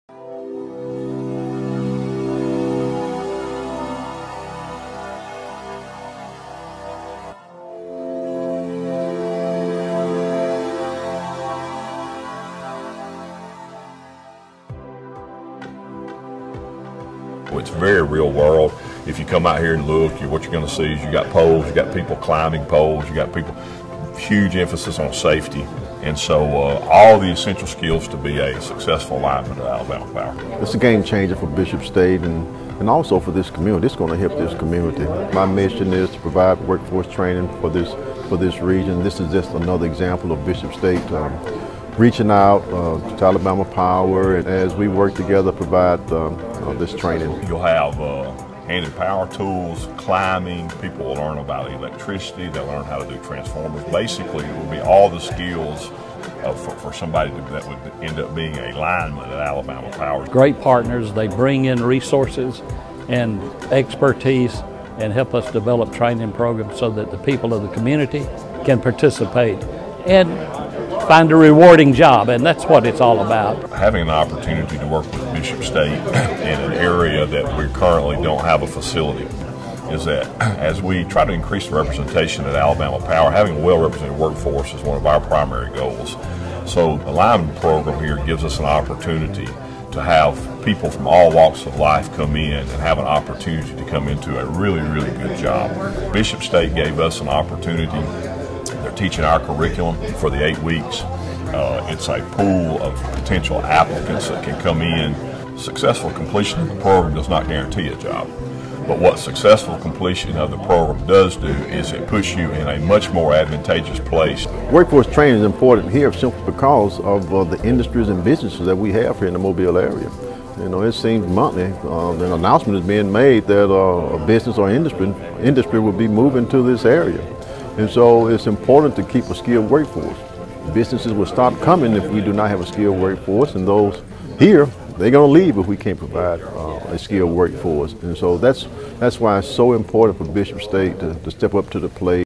Alabama Power pens lineman training agreement with Bishop State
A new Alabama Power Lineman Training Field on Bishop State Community College's campus is one of the Alabama Community College System's latest partnerships for workforce development. Bishop State will train electrical utility linemen for Alabama Power. The agreement was signed Thursday, April 4, by Jeff Peoples, Senior VP of Employment Services at Alabama Power, Dr. Reginald Sykes, President of Bishop State, and Jimmy Baker, Chancellor of the Alabama Community College System.